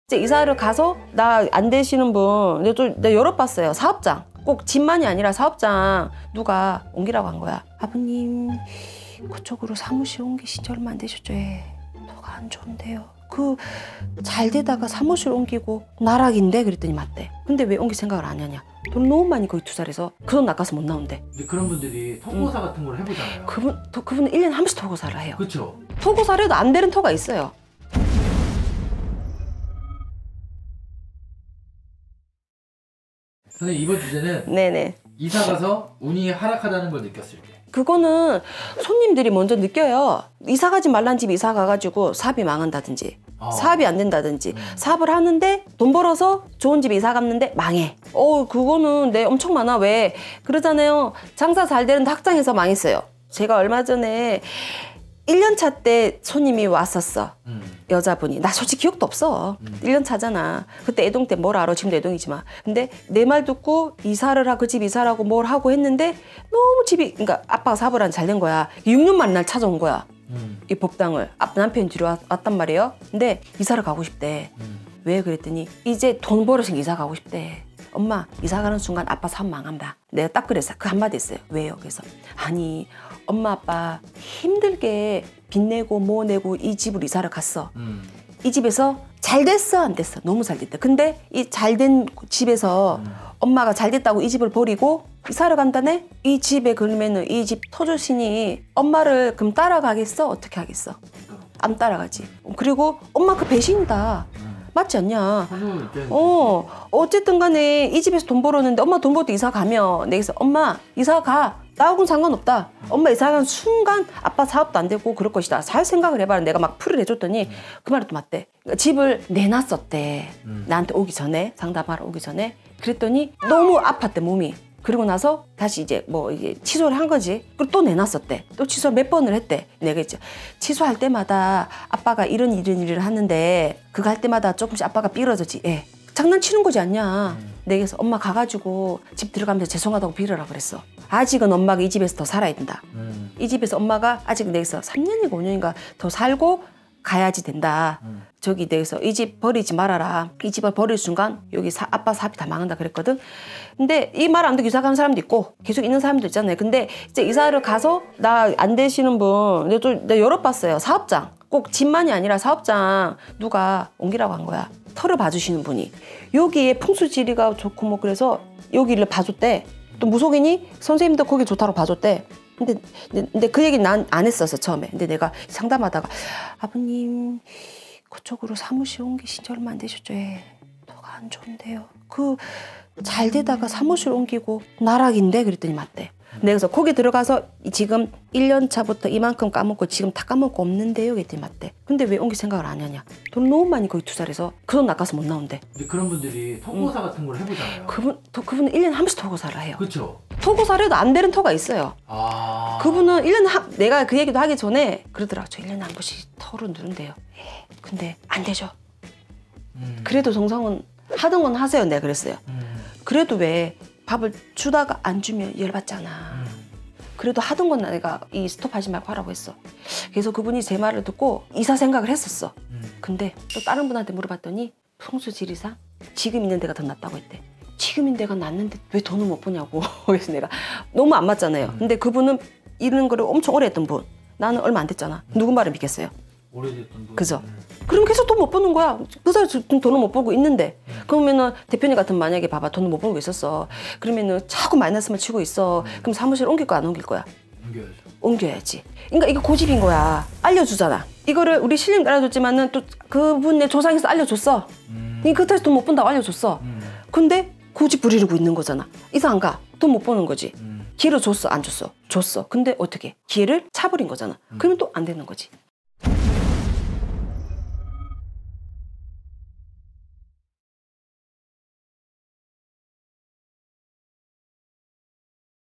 이상하게 이사 간 뒤로 되는 일이 없고 나쁜일만 일어난다면..?😱 새로운 터에서 터고사를 지내도 소용이 없는 까닭💥
★고은별신당

고은별신당 선생님은 덕분TV 소속 선생님이십니다

※고은별신당 선생님 지난 영상